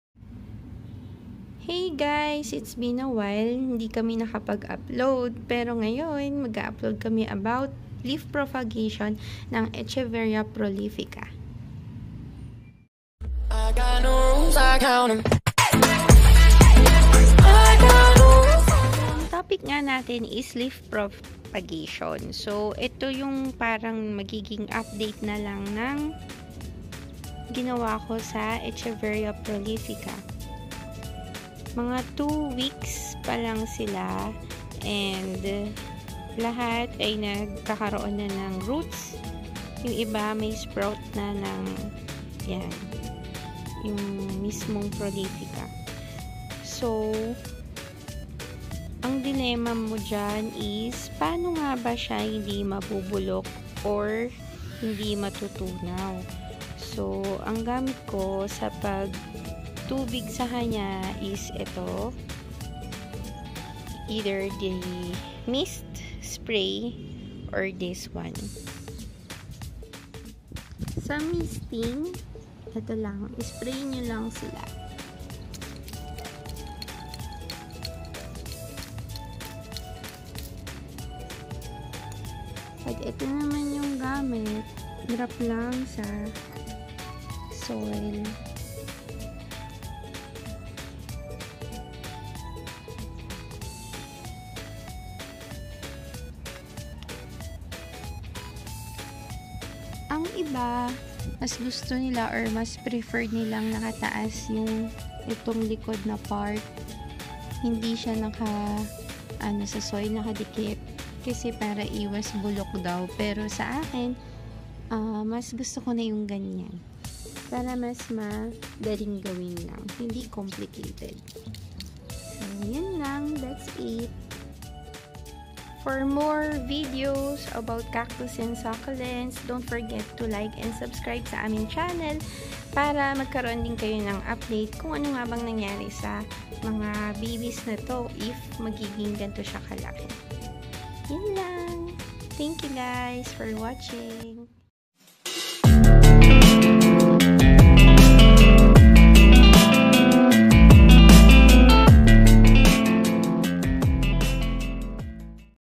Leaf Propagation - Echeveria Prolifica
A newbie keeper of cactus and succulents. Sharing you easy and simple steps in caring and propagating it.